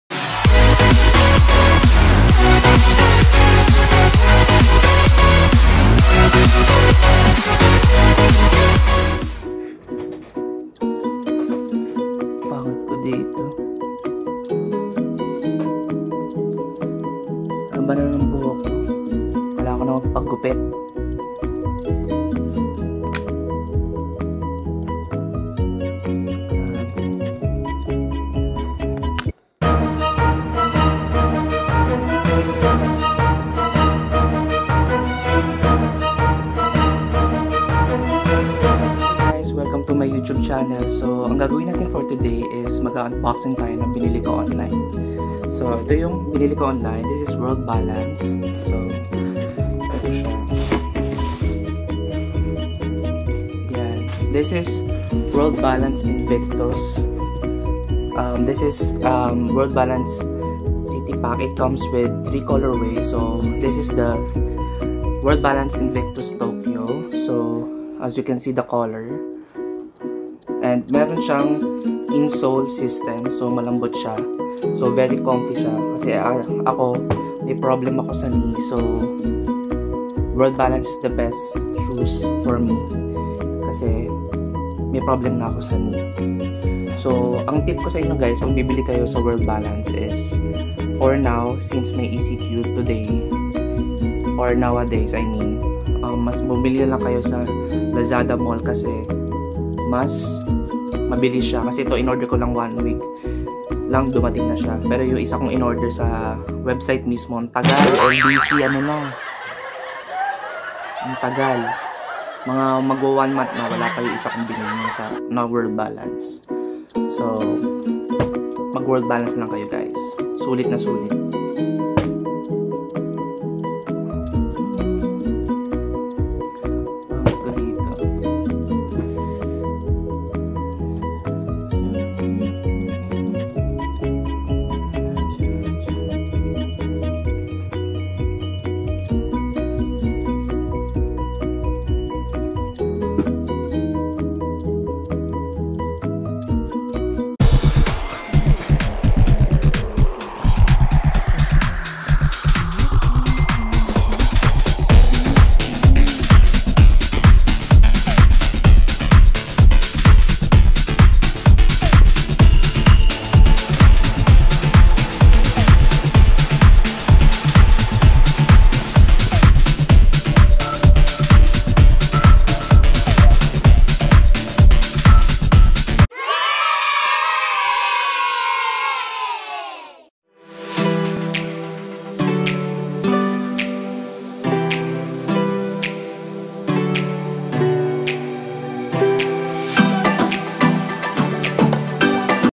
World Balance Unboxing [VLOG#1]
PAPOGING UNBOXING:

Also shared my experience ordering online (Website & Lazada). Sorry awkward pa sa camera haha.

Thank you Vlog Star App for my ending video.